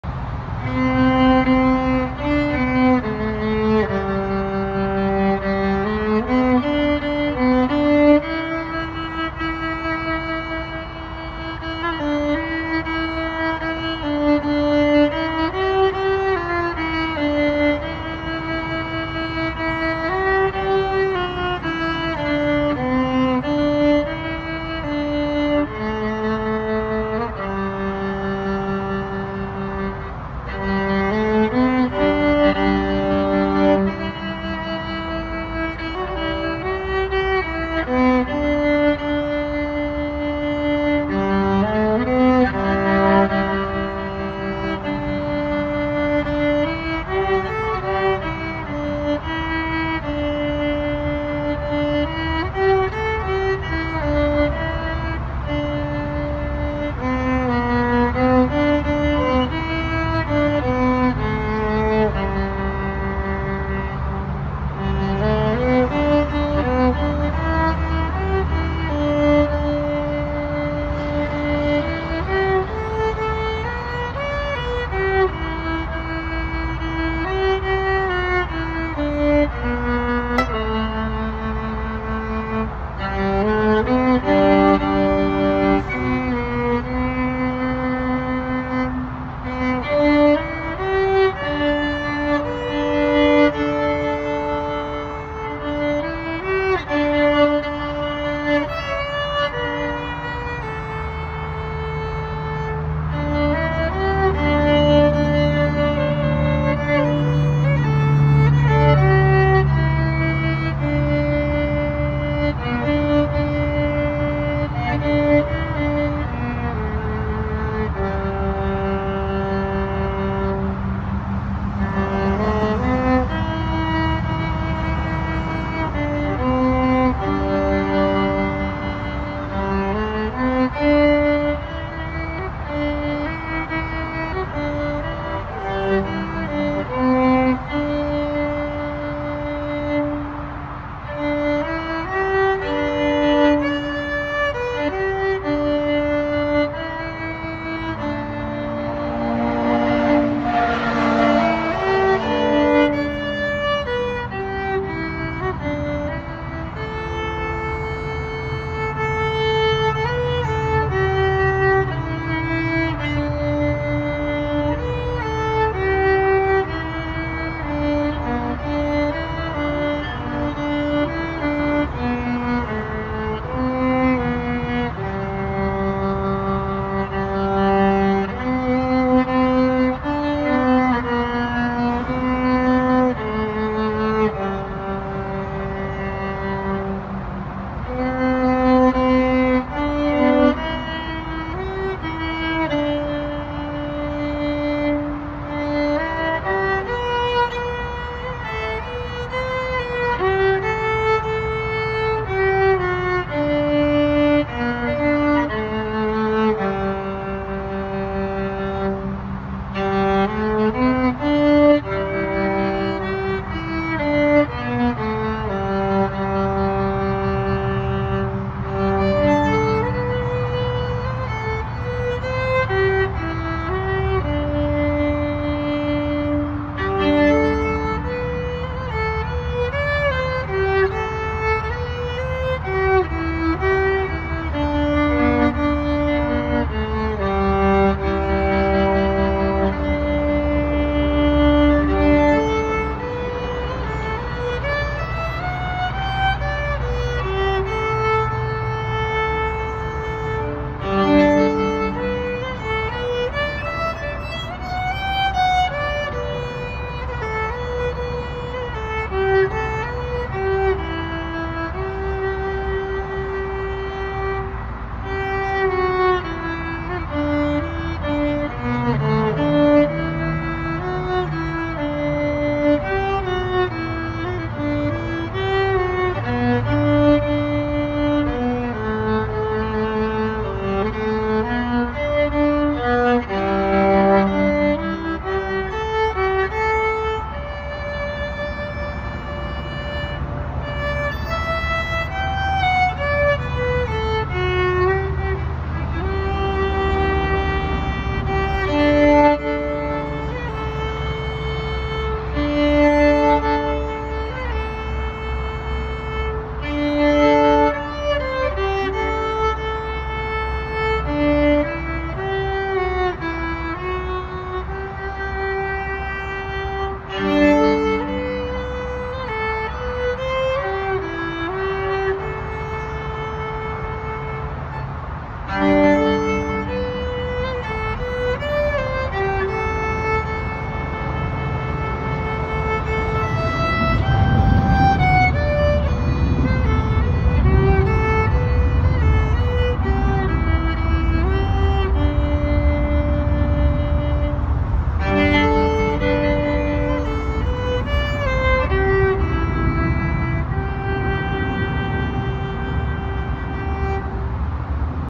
Worship flow Wednesday (4)
Free flow worship with my violin 🎻